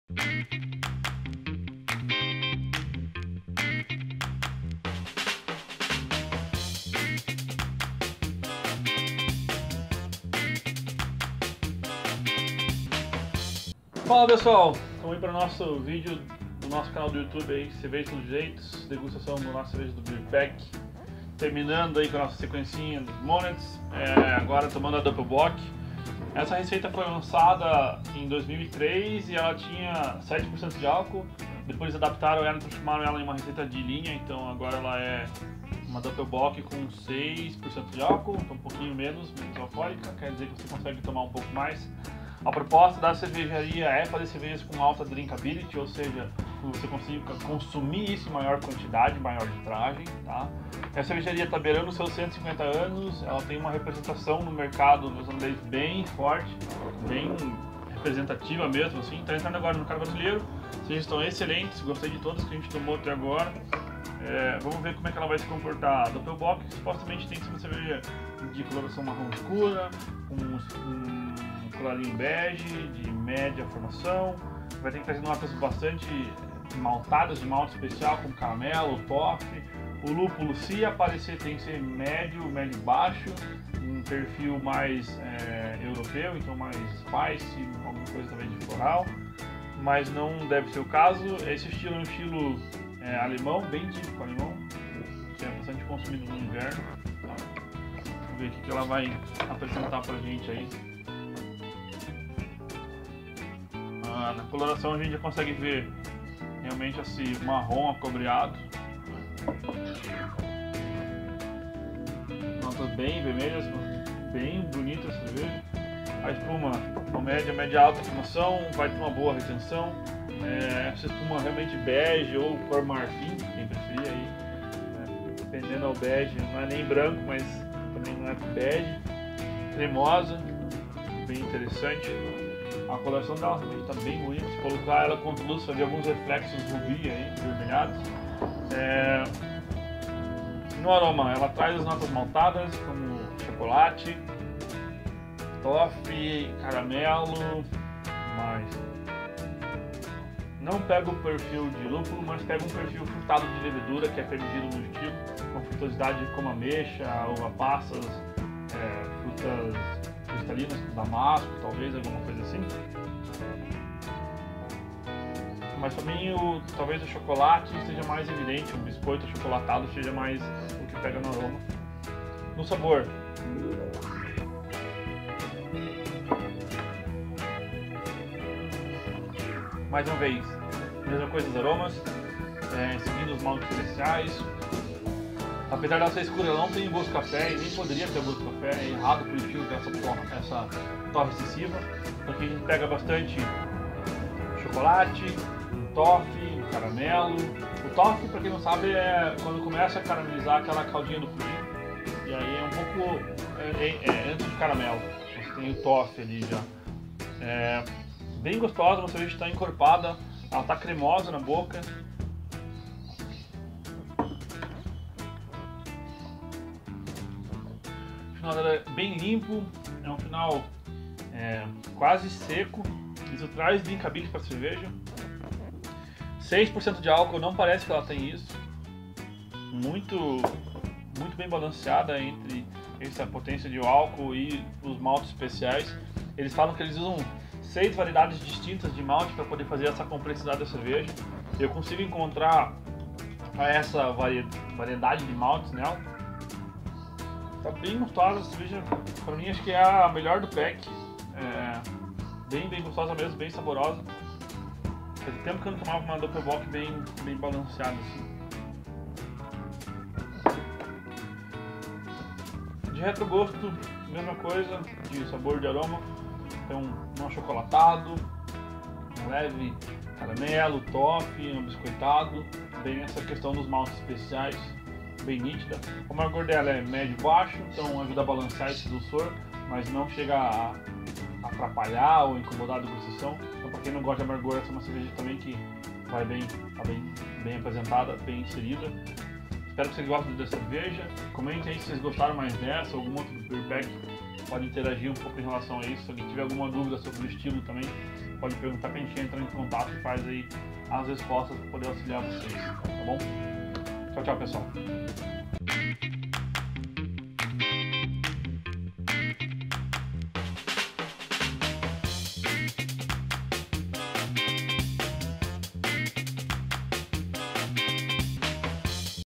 Monteith’s Doppel Bock - Degustação Beer Pack outubro 2016
No mês de outubro, de 2016, o Beer Pack trouxe quatro incríveis rótulos da Monteith’s Brewery, uma cervejaria da Nova Zelândia com aproximadamente 150 anos de experiência.
Confira a degustação da Monteith’s Doppel Bock, uma cerveja potente, excelente para acompanhar aquele churrasco de domingo.
Ainda não é assinante? Receba uma seleção de cervejas todos os meses na porta da sua casa